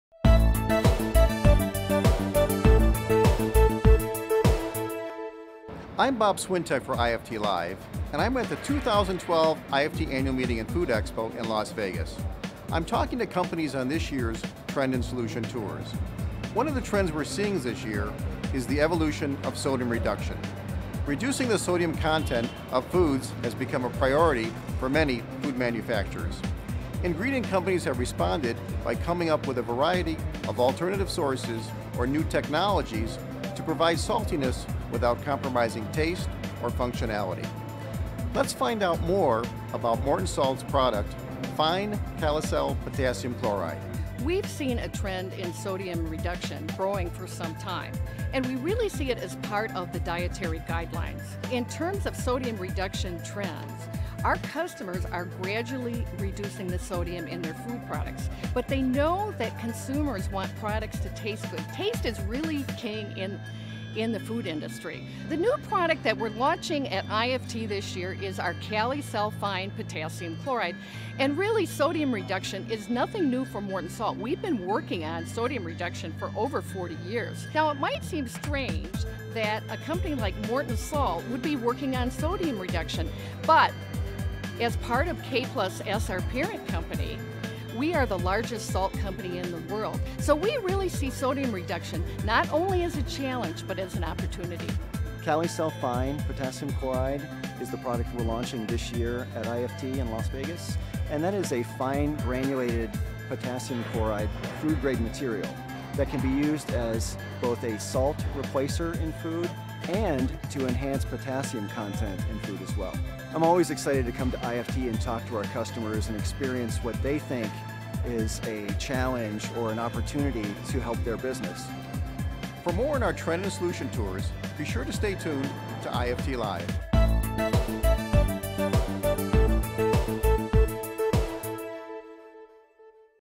IFT 12 Trend Tour: Morton Salt
IFT's Bob Swientek reports on one of the Trend and Solution Tours at the 2012 IFT Food Expo in Las Vegas. He spoke with Morton Salt's Linda Kragt and Chris Meyers about Fine KaliSel Potassium Chloride and why it's part of the Evolution of Sodium Reduction trend.